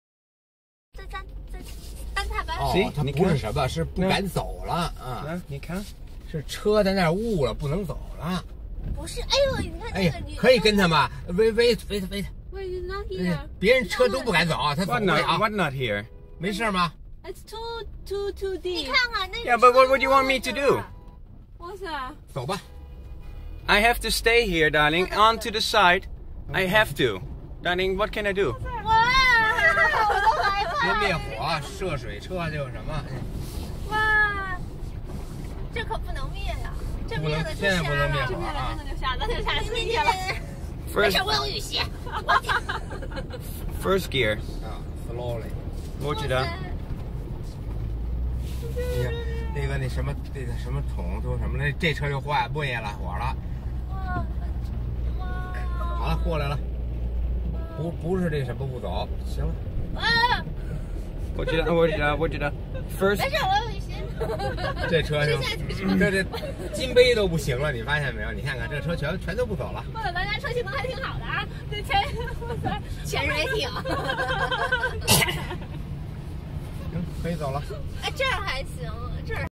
Heavy rainfall in Beijing, Shiliheqiao
26 August 2011

More than 30cm's of rain on the crossing of sanhuan and dayangfang at shiliheqiao.

People called me insane after seing this... and then confirmed it: 'Don't forget he's from Holland'!